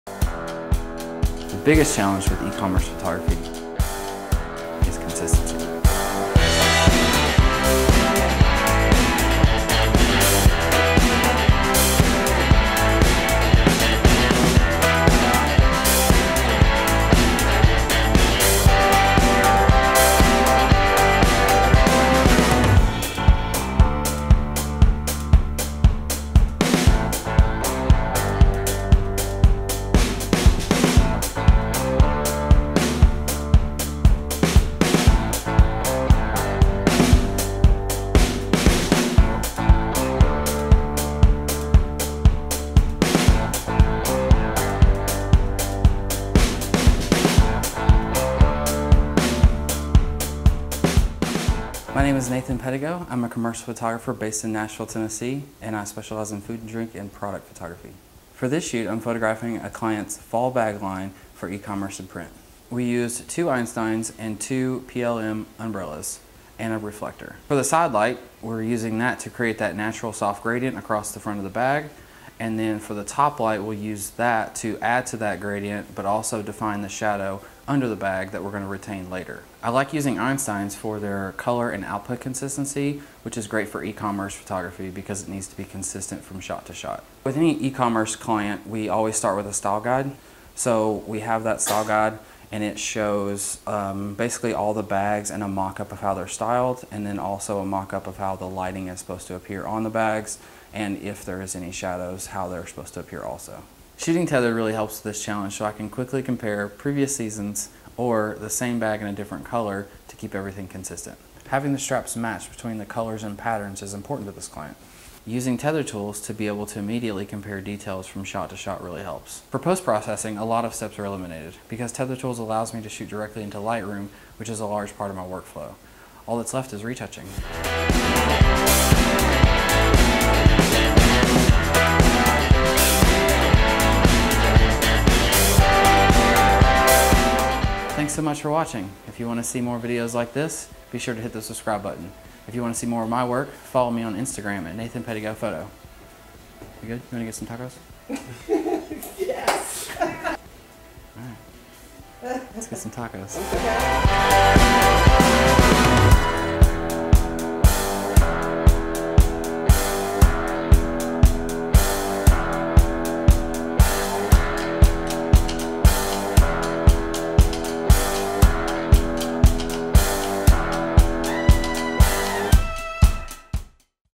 HOW I GOT THE SHOT | Nathan Pedigo and Tether Tools
Paul C. Buff, Inc. goes on set with Nashville-based e-commerce and food photographer, Nathan Pedigo, and Tether Tools. Nathan shares his process for shooting e-commerce images for a clients fall handbag collection.

FOLLOW PAUL C. BUFF